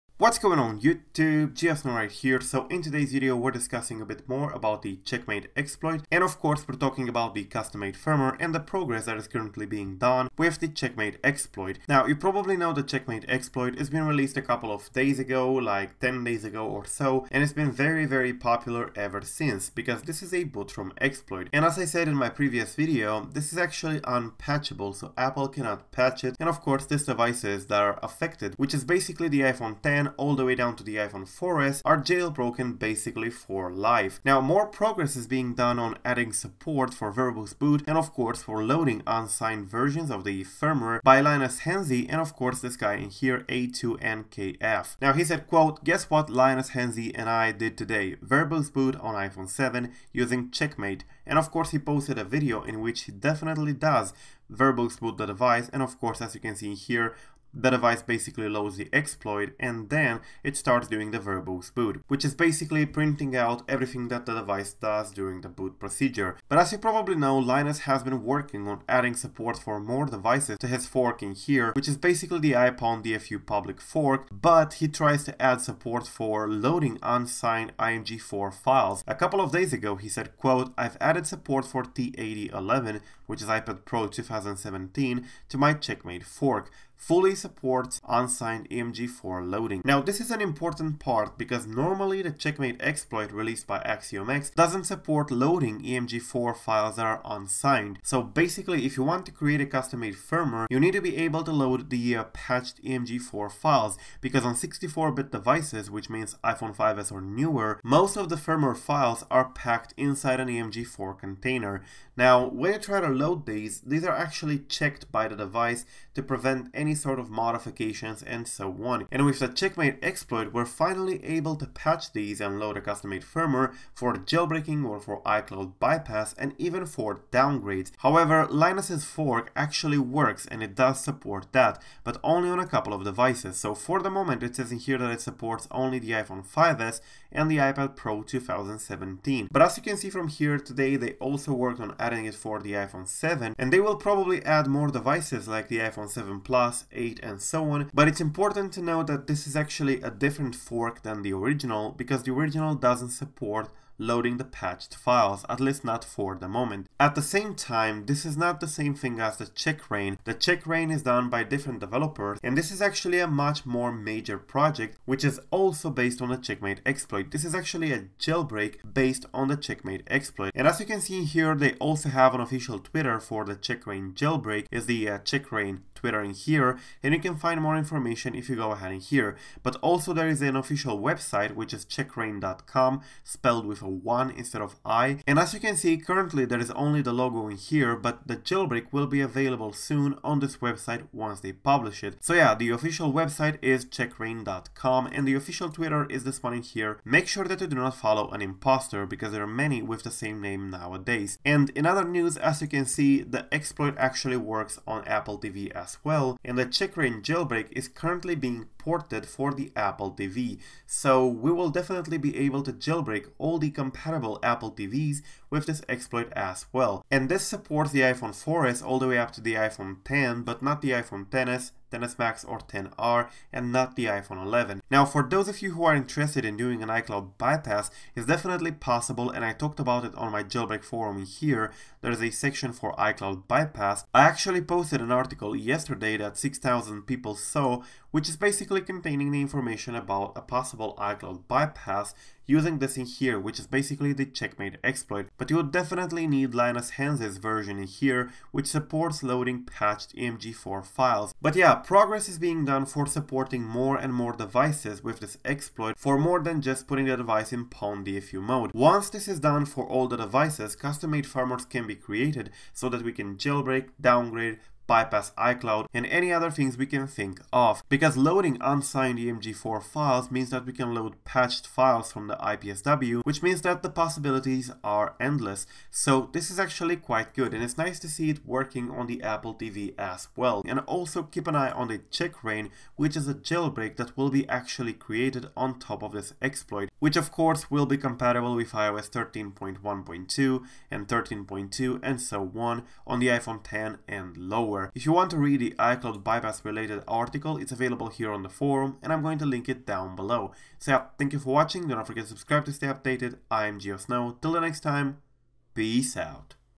iOS 13.1.2 / iOS 13 / 12 CheckM8 / CheckRa1n JAILBREAK / CFW News + PROGRESS
In today's video, we're discussing the current status of the CheckM8 BootROM exploit released a week and a half ago by developer @axi0mX, as well as the current status of CheckRa1n, a full Jailbreak solution developed by KJC team on top of the CheckM8 exploit. The CheckM8 exploit is an unpatchable SecureROM (BootROM) exploit for iPhone 4S all the way up to the iPhone 8 and iPhone X (and everything in-between), supporting all iOS versions that exist, or will exist for these devices, rendering a jailbreak forever. In this video, we talk about the progress being done to port the exploit features such as CFW creation to other devices beyond the initial support.

Of course, most developers in our community who started tackling this exploit quickly realized it's not an easy one. BootROM is the lowest level possible. This is written in the silicon chip and it cannot be updated by Apple. This is a very powerful exploit but with great power comes great responsibility. A Jailbreak or a CFW (Custom Firmware) made with this exploit would be tethered. It would require a program such as CheckRa1n to be started on the computer with the device in DFU Mode every time the person wants to enable the jailbreak or to boot the CFW.

Big progress is also being made to port CheckRa1n to Apple TV. In fact, Apple TV devices have similar SoCs which are containing a vulnerable version of the BootROM. This exploit has been patched in A12, so iPhone XS, iPhone XS Max, iPhone XR, iPhone 11 and most newer iPads except this year's iPad 7 (A10) are not vulnerable.

▽ Resources ▽
▶ How To Use CheckM8 BootROM Exploit For CFW / Jailbreak (iOS 13, iOS 12, iOS 11 PWNED DFU Mode)